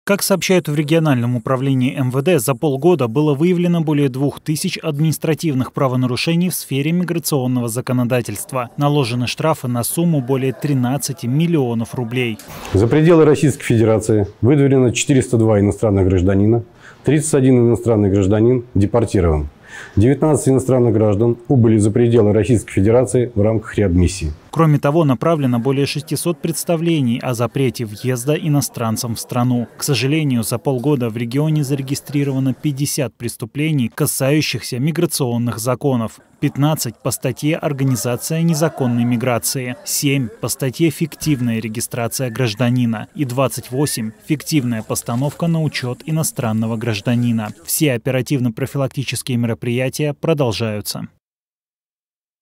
Более 400 мигрантов из Смоленской области выдворили за пределы страны
В Смоленской области полицейские провели профилактические рейды по выявлению фактов нарушения миграционного законодательства.

Принято решение о выдворение 402 иностранцев за пределы Российской Федерации.

На территории Смоленской области с начала года проведено более 2 тысяч профилактических мероприятий, направленных на выявление фактов нарушения миграционного законодательства. Осуществлена проверка мест пребывания и осуществления трудовой деятельности иностранных граждан.
В результате проведенных мероприятий выявлено свыше 2 тысяч административных правонарушений в сфере миграционного законодательства, предусмотренных главой 18 КоАП РФ.  На нарушителей административного законодательства наложены штрафы на сумму, превышающую 13 млн рублей.
За пределы Российской Федерации выдворено 402 иностранца, 31 иностранный гражданин депортирован, 19 лиц убыли за пределы страны в рамках реадмиссии.
В МВД России полицейскими направлено свыше 640 представлений о неразрешении въезда иностранным гражданам и лицам без гражданства в Российскую Федерацию.
В регионе зарегистрировано 50 преступлений:
– 15 по статье 322.1 УК РФ «Организация незаконной миграции»;
– 7 по статье 322.2 УК РФ «Фиктивная регистрация гражданина Российской Федерации по месту пребывания или по месту жительства в жилом помещении в Российской Федерации и фиктивная регистрация иностранного гражданина или лица без гражданства по месту жительства в жилом помещении в Российской Федерации»;
– 28 по статье 322.3 УК РФ «Фиктивная постановка на учет иностранного гражданина или лица без гражданства по месту пребывания в жилом помещении в Российской Федерации».
В настоящее время проведение оперативно-профилактических мероприятий продолжается.